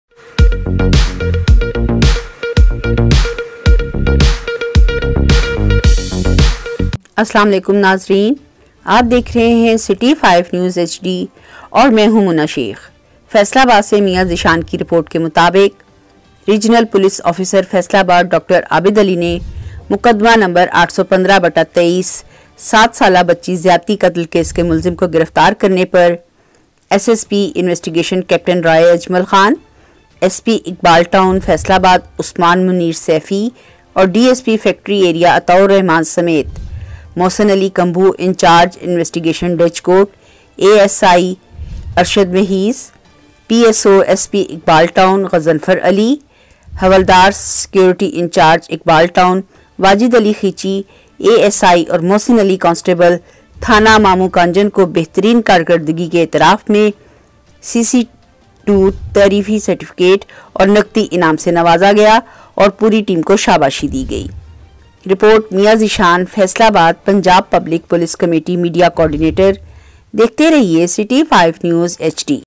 Breaking news Police Officer Faisalabad Dr. Abid Ali SSP Investigation Captain R. Roy Ajmal Khan
Police Officer Faisalabad Dr. Abid Ali SSP Investigation Captain R. Roy Ajmal Khan, SP Iqbal Town Faisalabad Usman Munir Safi and DSP for arresting the accused in Case No. 815/23 seven-year-old girl rape and murder case.  Factory Area Atta-ul Rehman Sumit Mohsin Ali Kamboh In-charge Investigation Dijkot, ASI Arshad Mahis PSOSP Iqbal Town, Ghazanfar Ali Havaldar Security In-charge Iqbal Town, Wajid Ali Khichi ASI and Mohsin Ali Constable Police Station Maman Kanjan for excellent performance.  In recognition, CC2 was awarded a Certificate of Appreciation and a cash prize and felicitated the entire team

 Report Mian Zeeshan Faisalabad Punjab Public Police Committee Media Coordinator